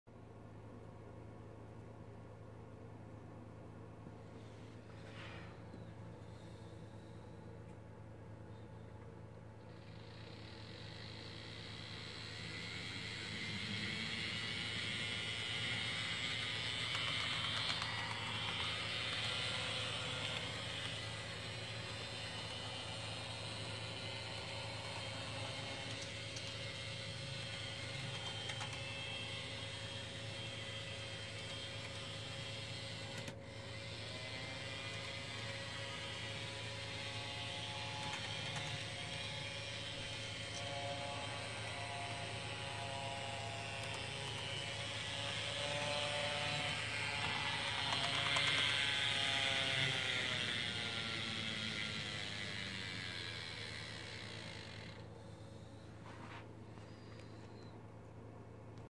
Ticket #181143
🚂🔧 Back on the Rails! 🌟 The Dapol N Gauge Class 67 triumphantly returns after warranty repair, undergoing successful testing in DC mode. Witness its revived power on the tracks!